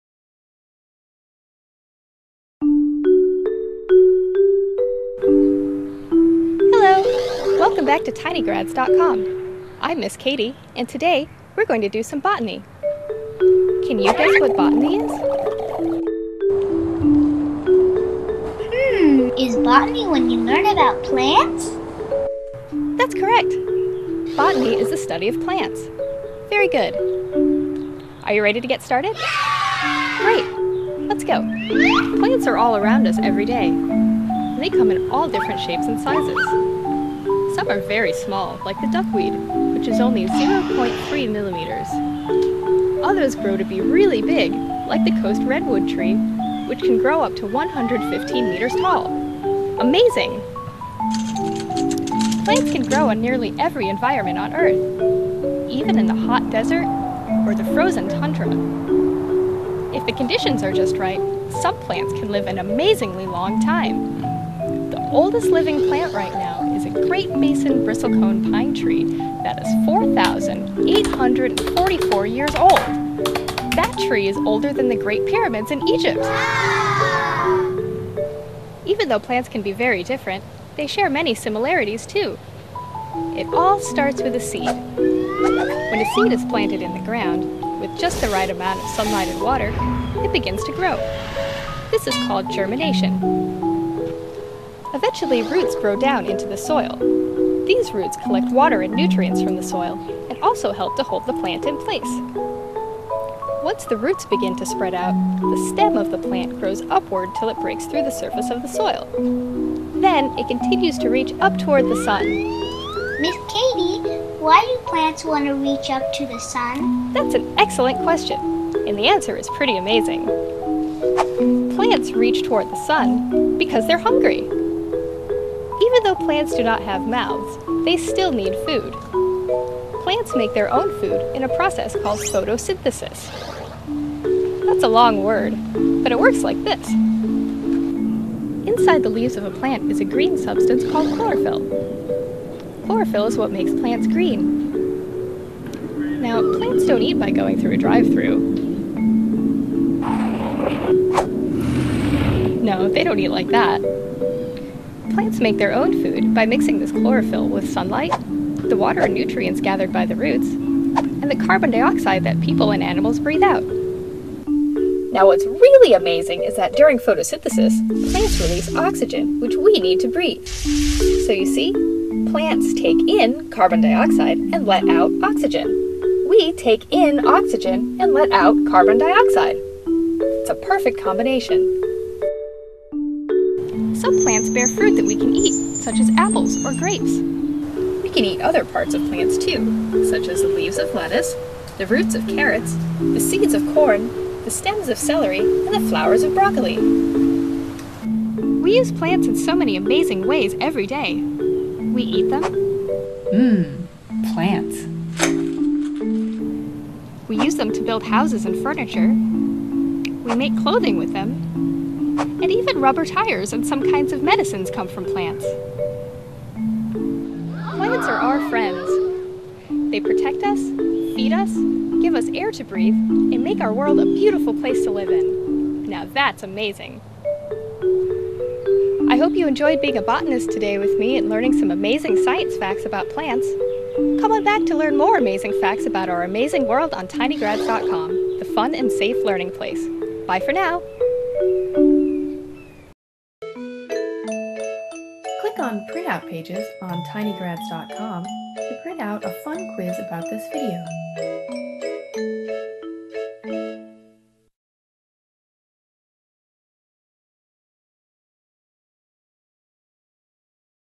Amazing Plant Facts! Fun Science Facts for children. Homeschool family.
Fun and Amazing Science facts with TinyGrads. Learn fun facts about animals.

Fun facts about plants are a pleasure to learn on Amazing Science with Ms. Katie.This episode is full of Interesting facts about plants. TinyGrads.com - The Fun and Safe Learning Place.